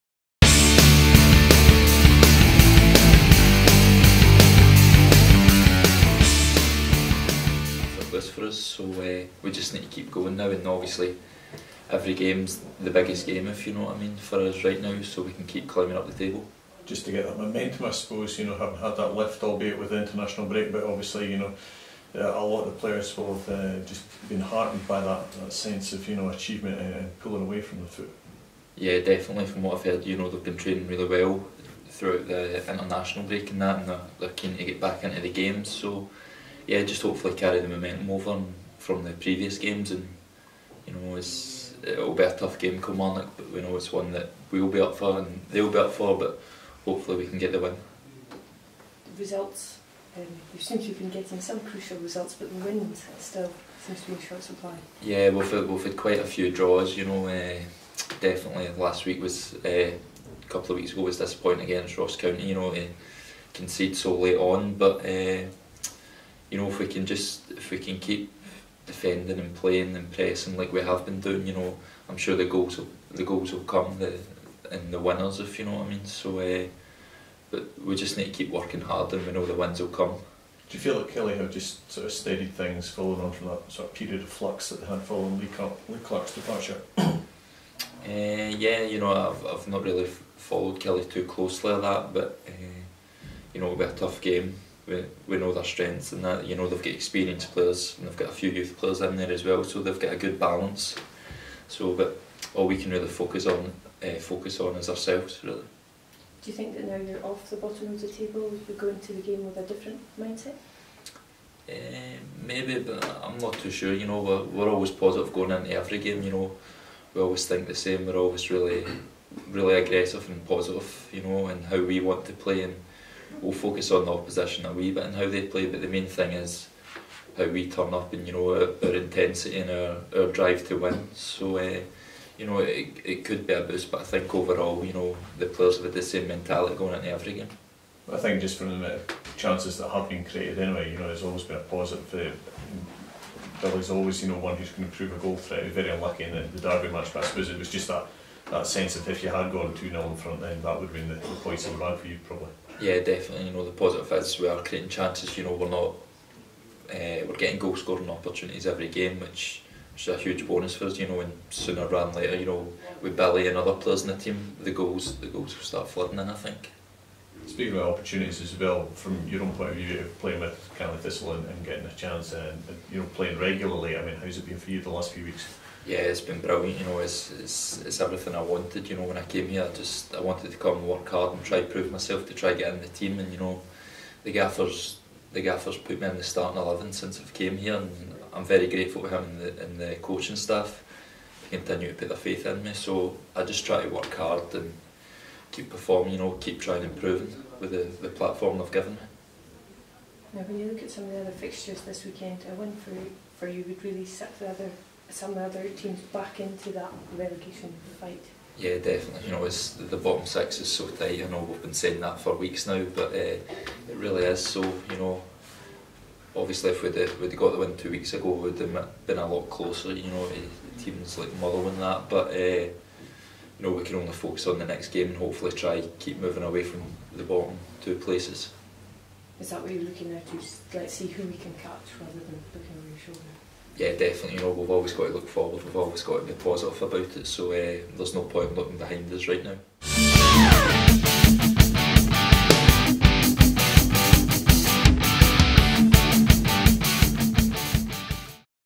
CaleyJagsTV : Jamie McCart Match Preview v Kilmarnock : 30/03/17
On Loan, Jamie McCart, talks to the press ahead of the weekend's Ladbrokes Premiership match v Kilmarnock in Inverness.